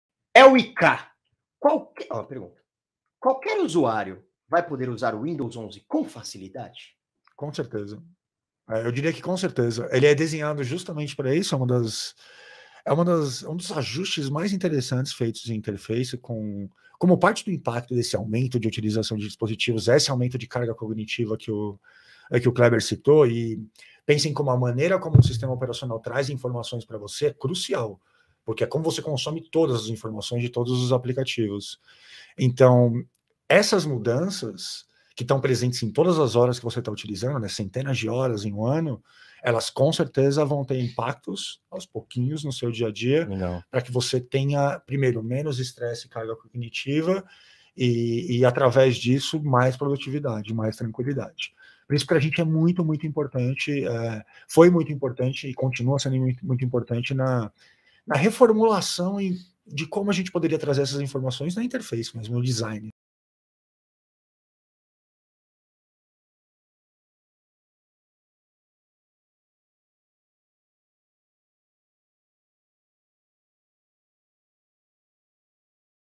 A maneira como o sistema traz informação é crucial! | Cortes Scancast
Com o novo lançamento da Microsoft, o Windows 11, o que muda em termos de segurança para os usuários?

O Windows 11 foi criado para o trabalho híbrido. A versão mais produtiva e segura do Windows, porém simples para a TI.